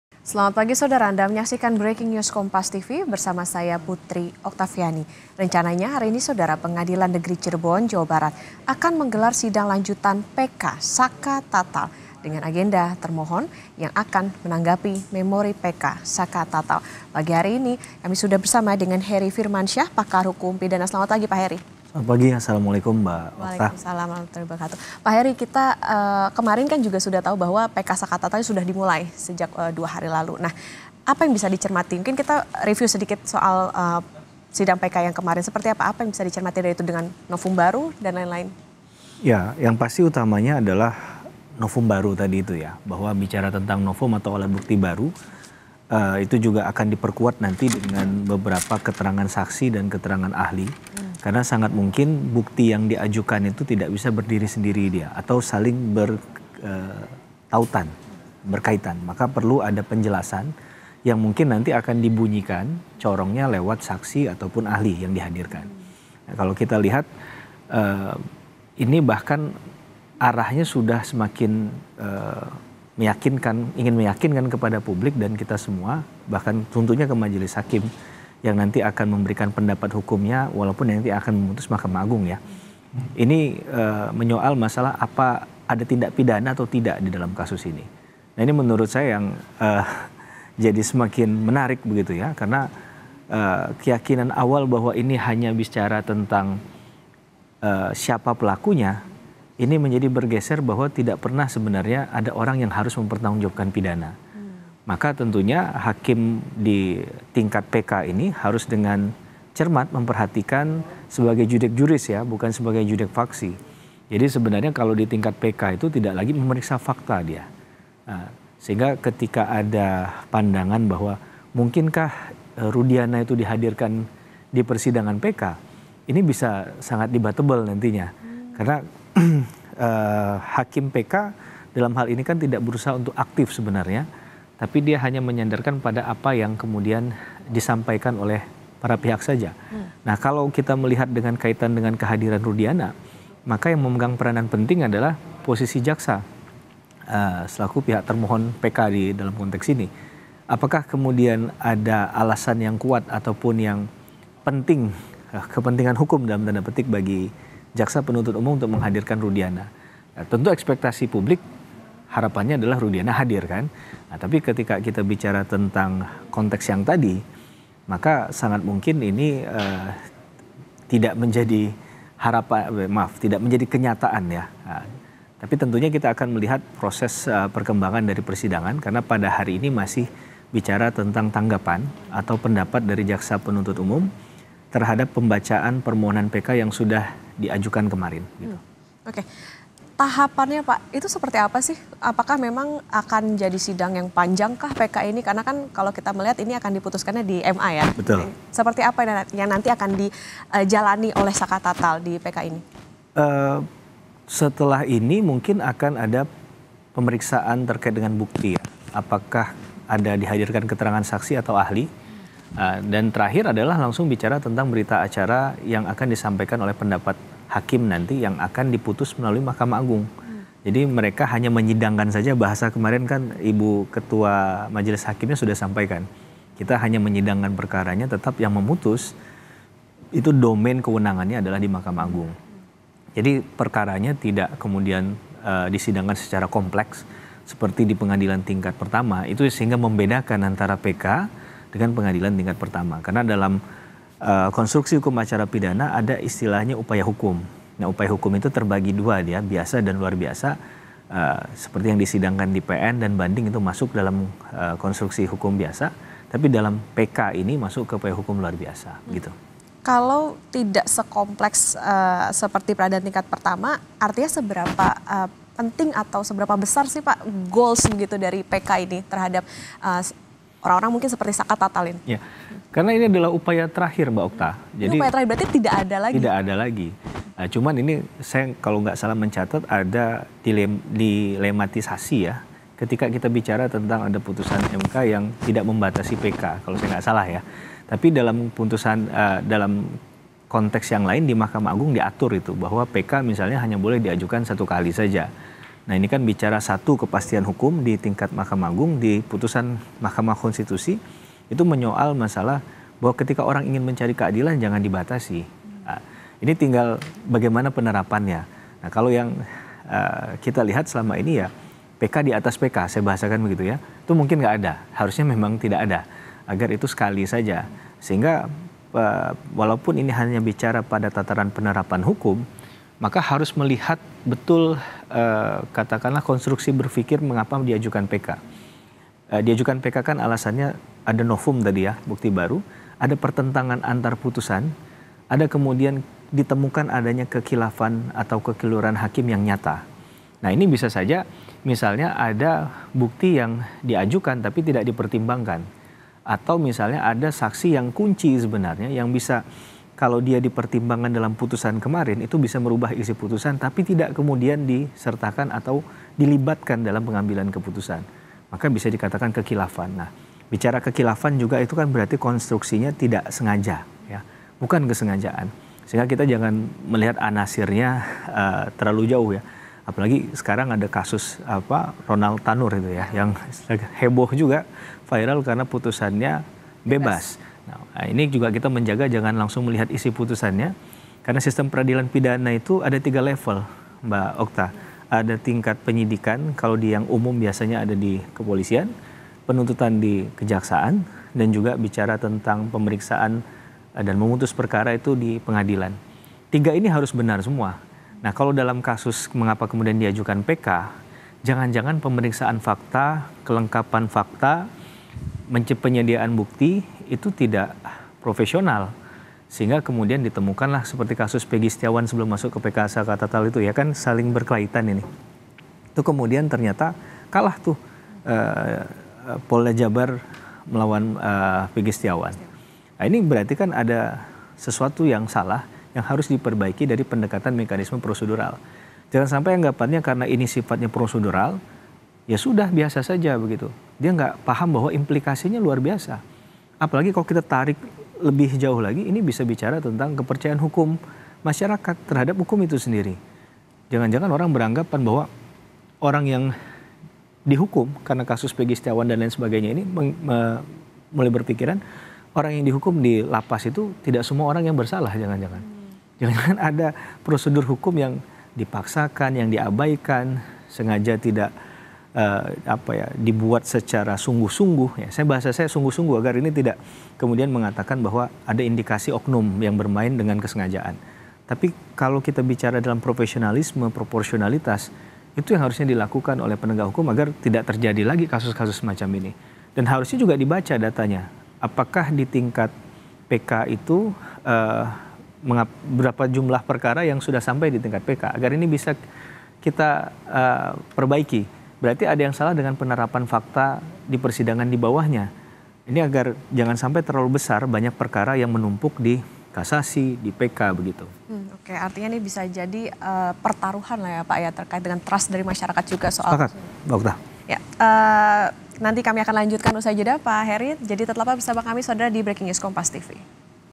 SANGAT Ditunggu! Sidang Lanjutan PK Saka Tatal Hari Ini, Termohon Siap?
KOMPAS.TV - Sebelumnya, sidang peninjauan kembali saka tatal di kasus vina Cirebon dilaksanakan pada Kamis (25/07).

Hari ini, sidang lanjutan PK Saka Tatal akan kembali dilakukan dengan agenda penyampaian tanggapan dari termohon atas memori PK Saka Tatal.

Menunggu persidangan dimulai, simak pembahasan Kompas TV bersama dengan Hery Firmansyah, Pakar Hukum Pidana Universitas Tarumanagara.